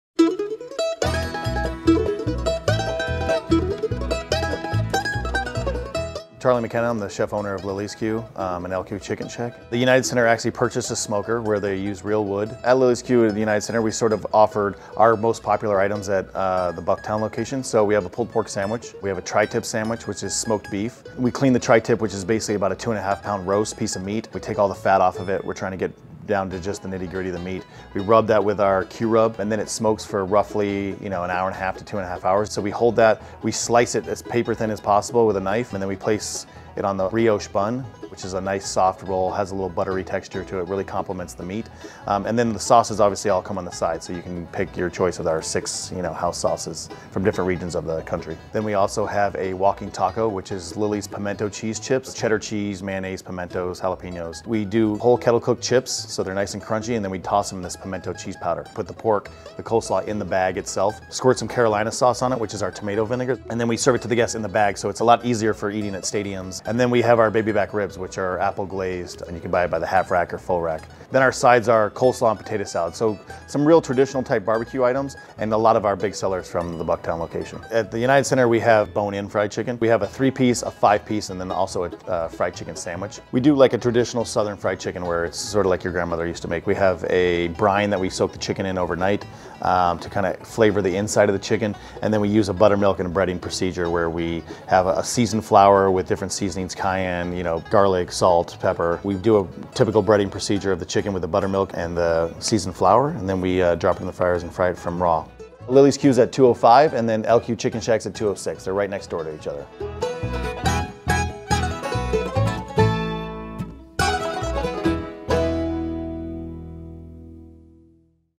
Chicago's Plate: Lillie's Q/LQ Shack
Chef/Owner Charlie McKenna describes the menu at Lillie's Q and LQ Chicken Shack at the UC.